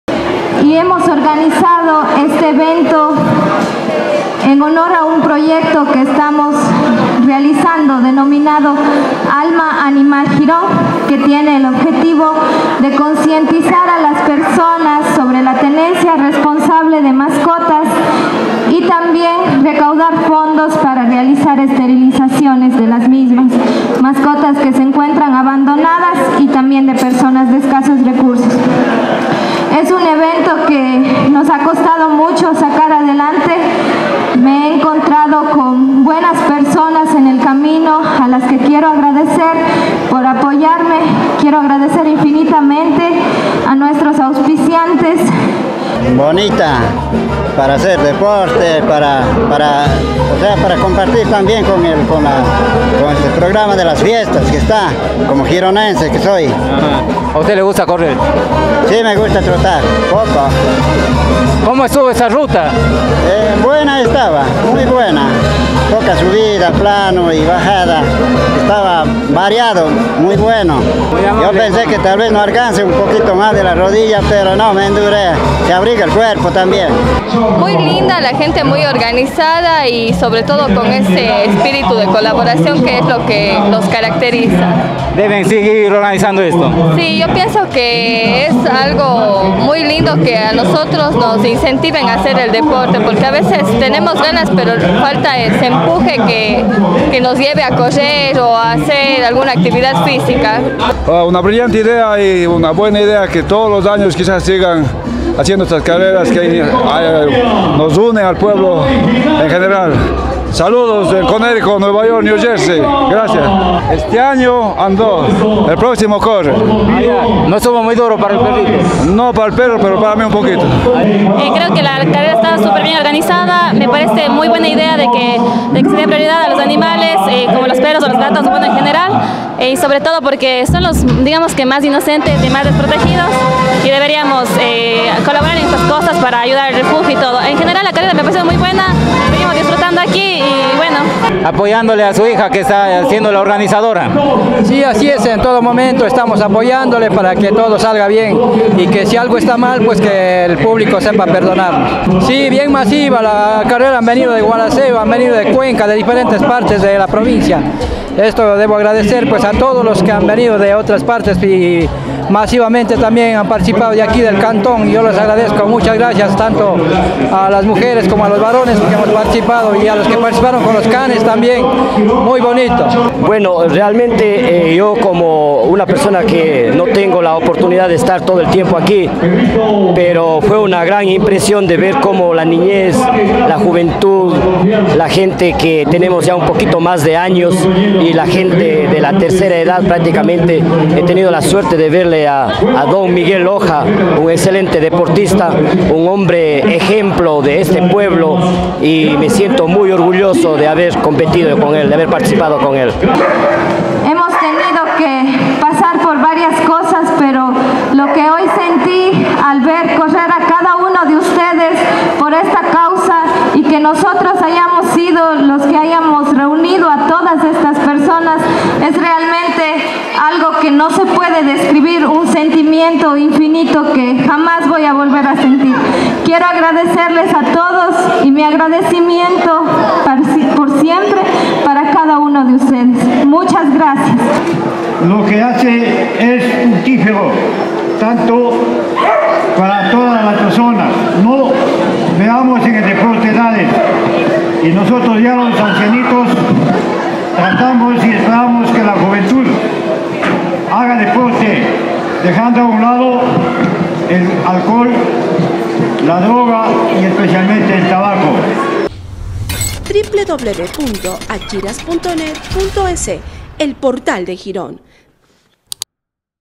Alma Animal recibió apoyo de deportistas y ciudadanos
280 atletas aceptaron la invitación de Karina Sánchez, cholita gironense para correr los 5 kilómetros alrededor de Girón en apoyo a su proyecto “Alma Animal”, con el cual  pretende iniciar una atención efectiva a la fauna urbana (perros y gatos) y las consabidas consecuencias.